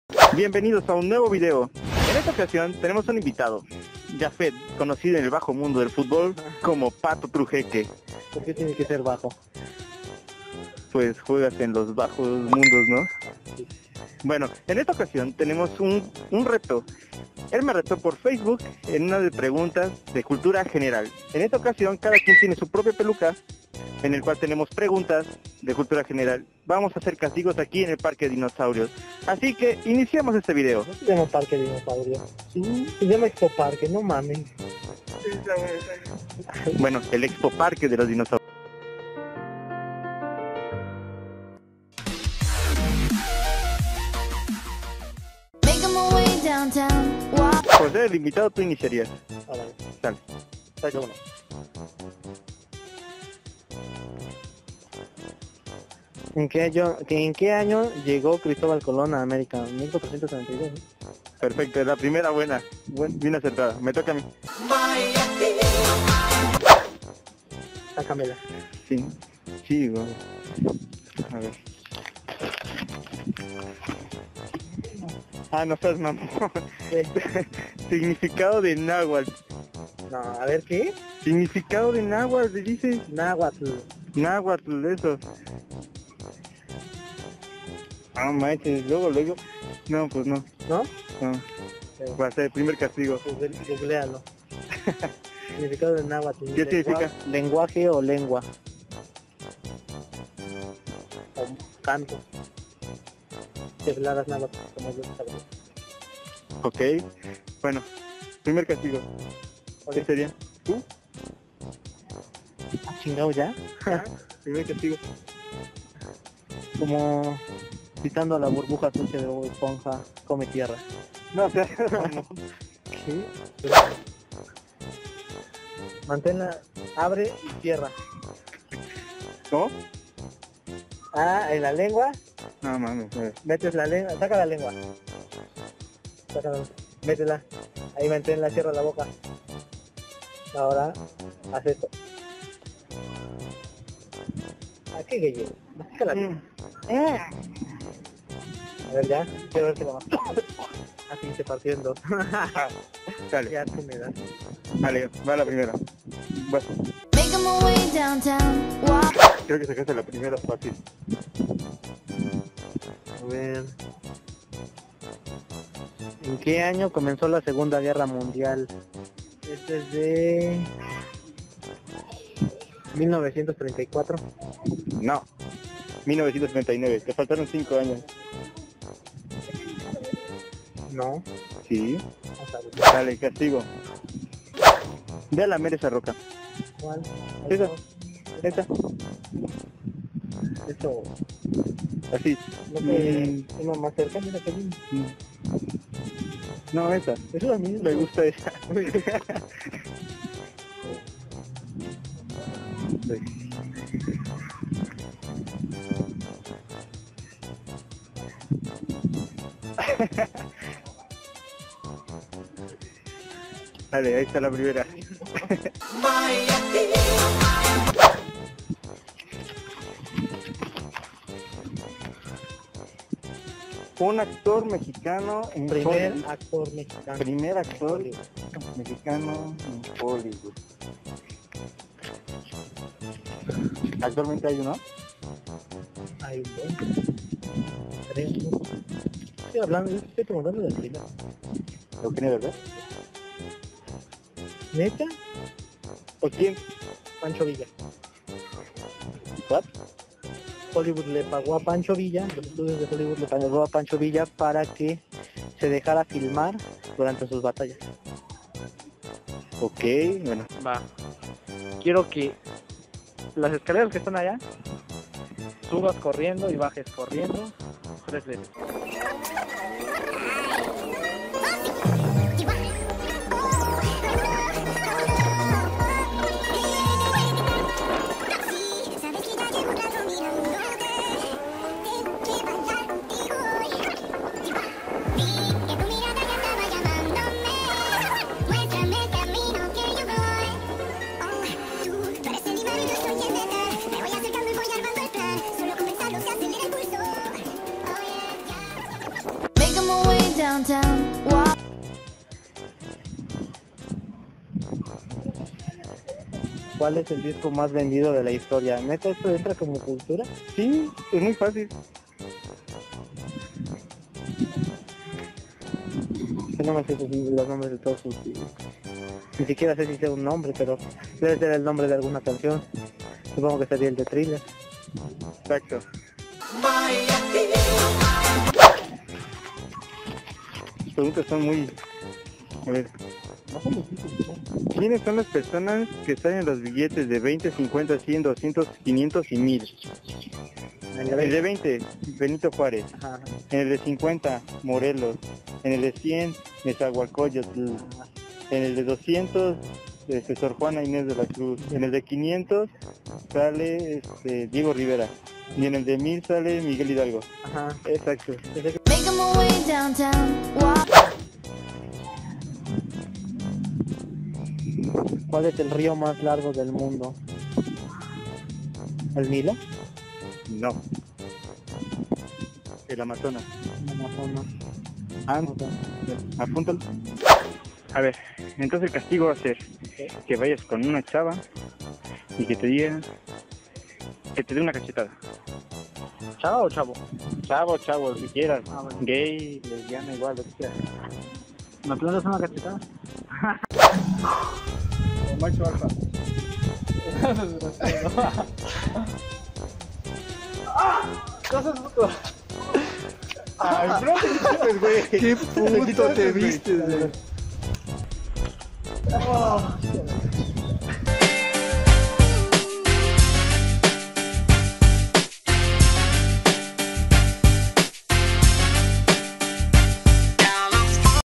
RETOS EXPO PARQUE ORIZABA | PREGUNTAS DE CULTURA GENERAL 2 PARTE 1 | PUEBLO MAGICO
ESPERO LES GUSTE LA PRIMERA PARTE ESPEREN LA SEGUNDA ESTA SEMANA!

◄USTEDES DÍGANME EN LA CAJITA DE COMENTARIOS ¿QUE TE PARECEN ESTAS NOTICIAS? SI QUIERES VER MAS VLOGS DEJAMELO SABER EN LA CAJITA DE COMENTARIOS.►